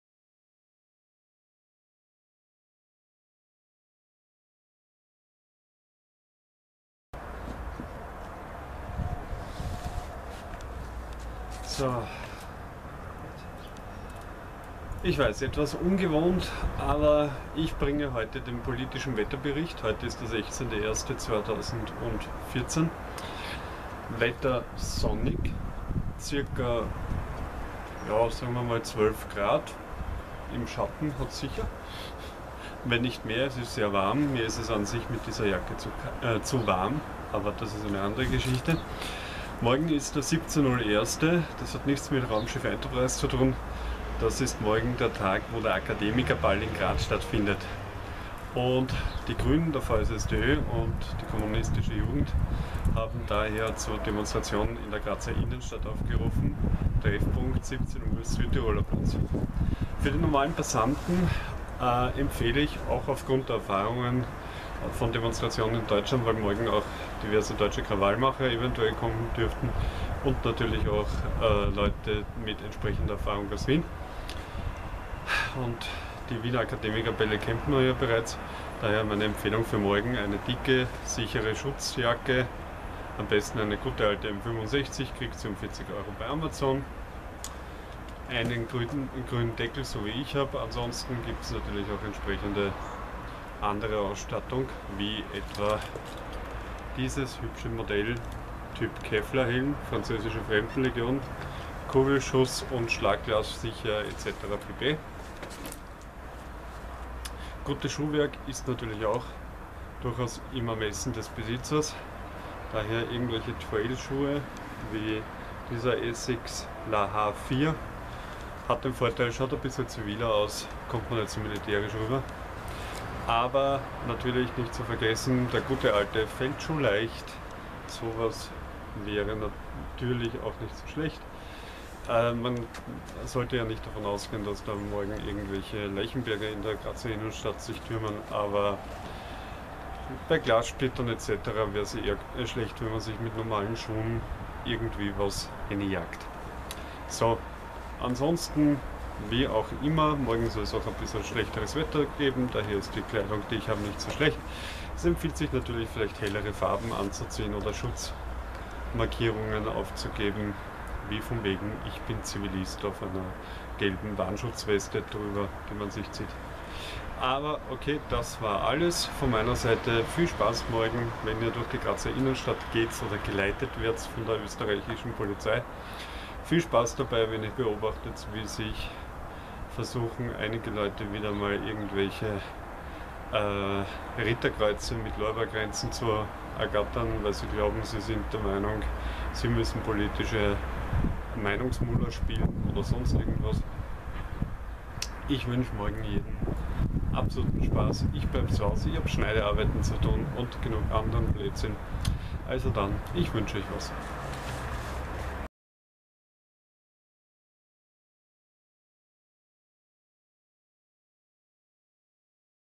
Inside Politics politischer Wetterbericht 16.1.2015
Einen Tag vor dem Grazer Akademikerball und der Demonstration dagegen, erklärt Sivic wie man sicher an Absperrungen und Demos vorbei kommt. Und ja ich hab beim Datum 2014 gesagt, sorry...